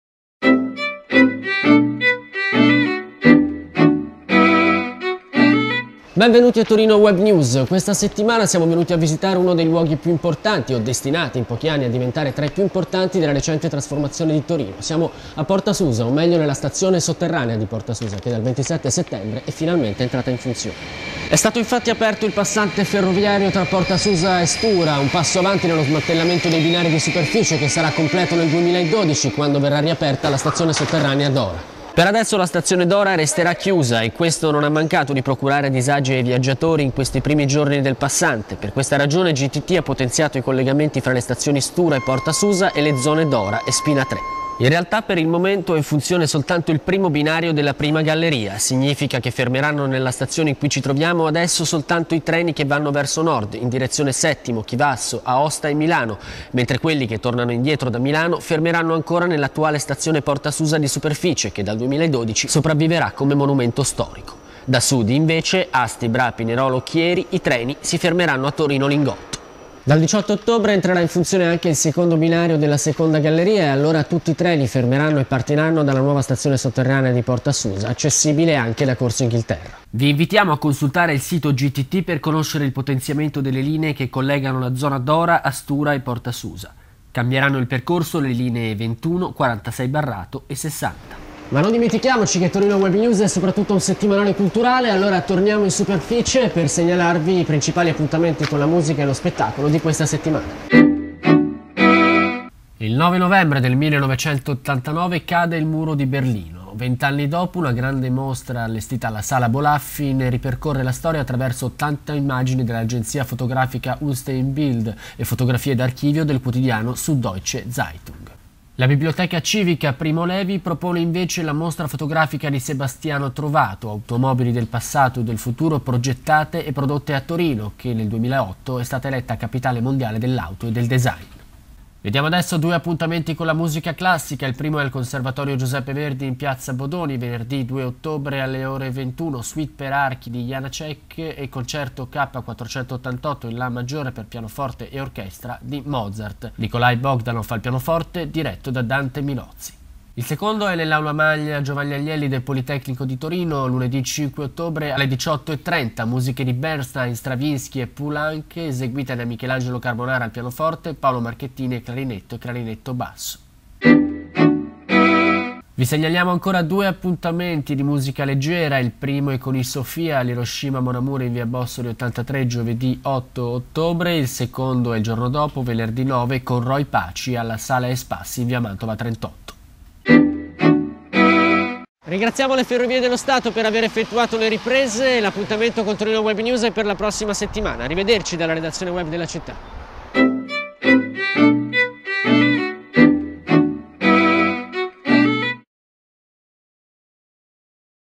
TWN 69 - Ed del 02/10/2009 - Porta Susa
Appuntamento settimanale con l'arte e la cultura in città. Il notiziario è stato girato alla stazione sotterranea Porta Susa in occasione dell'apertura del passante ferroviario tra Porta Susa e Stura. La trascrizione della puntata è disponibile all'indirizzo Per attivare i sottotitoli cliccare sulla freccia in basso a destra sul player.